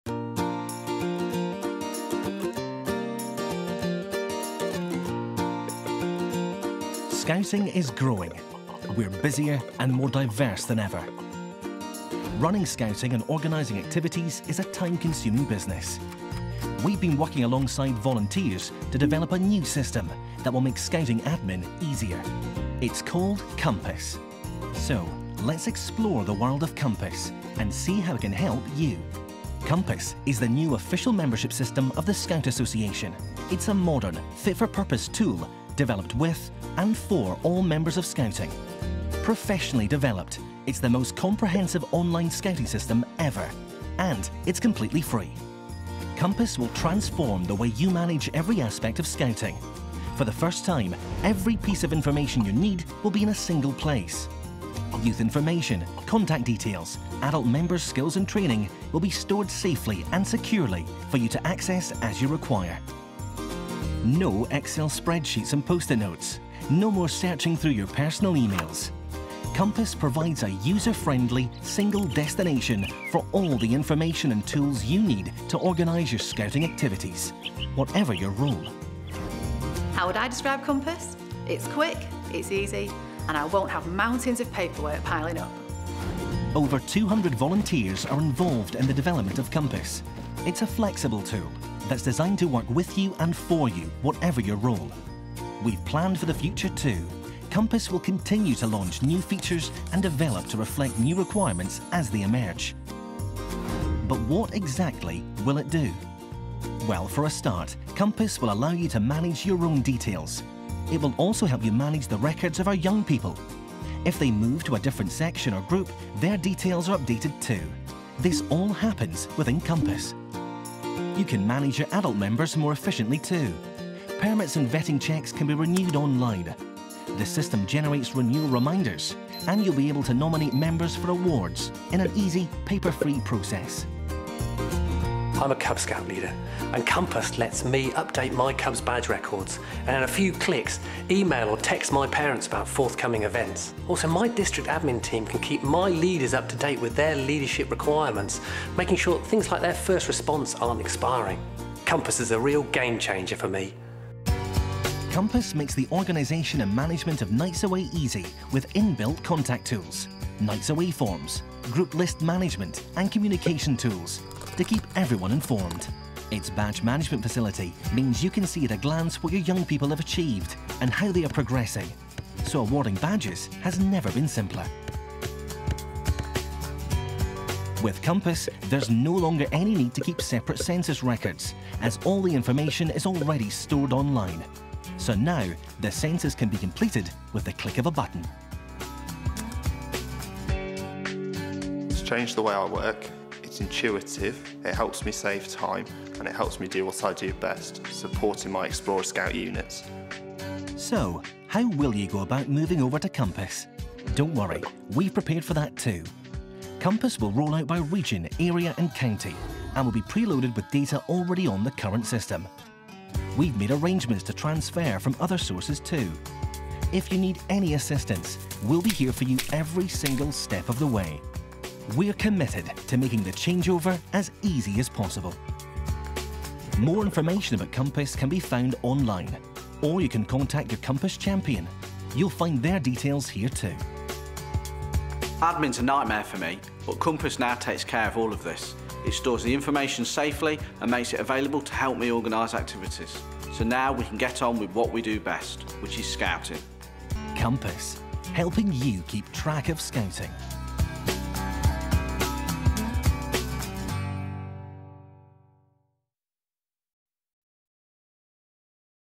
Scouts 'Compass'
The Scouts Organisation asked us to create an online piece for Compass - their new database and organisational software.  In a very tight timeframe we scripted, shot, animated and post produced this film.  Thankfully, we were prepared!